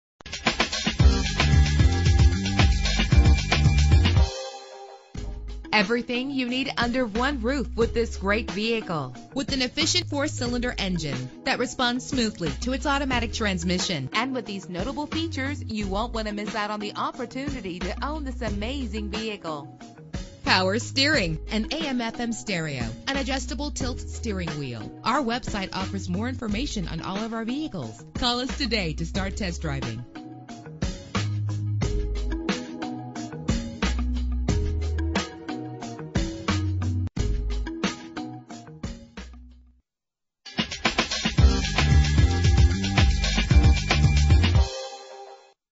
2000 Toyota Camry - Little Rock AR
Year: 2000
Make: Toyota
Model: Camry
Trim: LE
Engine: 2.2 liter inline 4 cylinder 16 valve
Transmission: 4-Speed Automatic
Color: Gray
Mileage: 212779
Address:
10825 Colonial Glenn Road